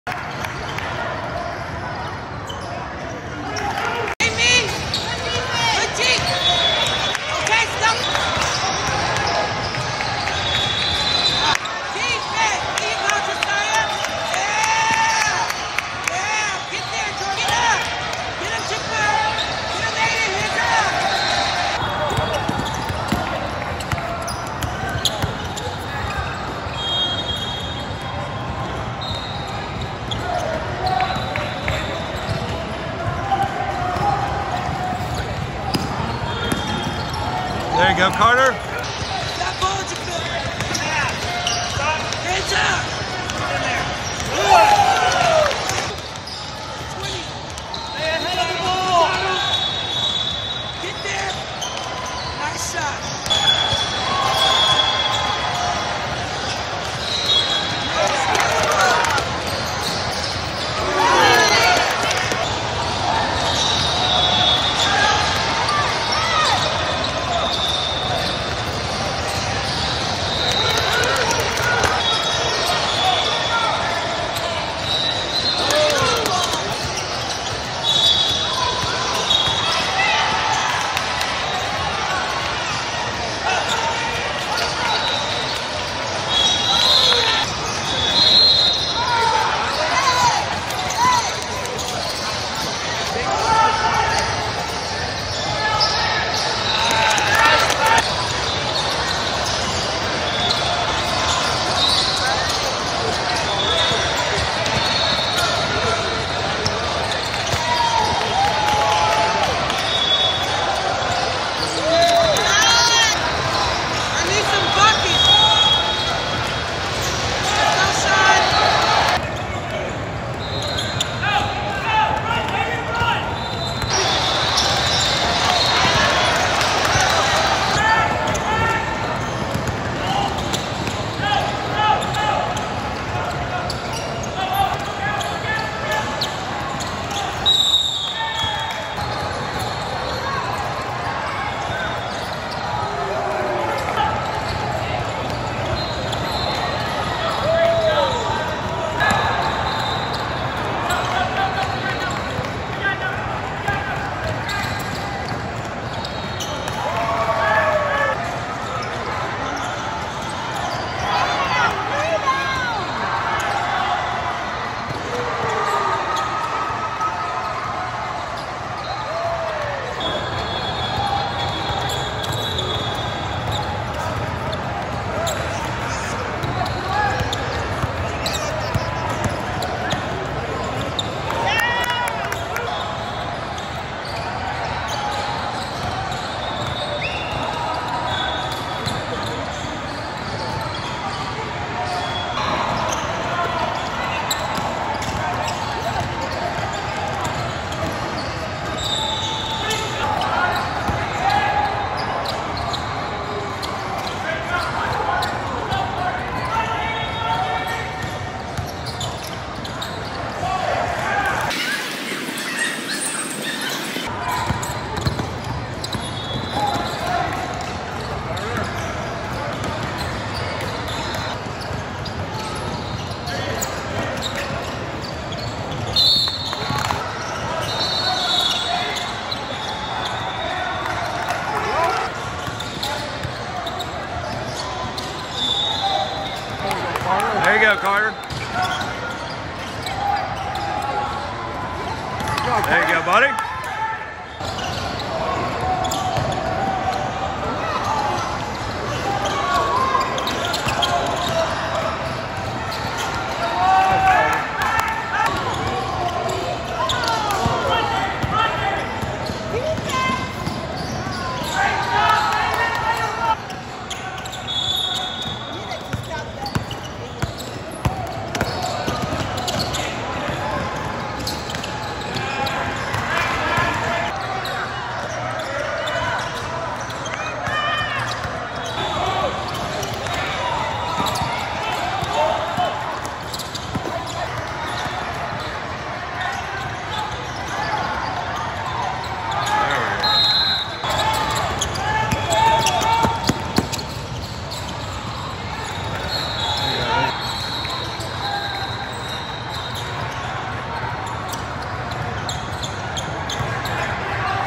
HoopSeen 2023 Best of the South Highlights
Carter had a great tournament! Averaged 9.4 PPG (47 points) on 11-27 3-point shooting (41%)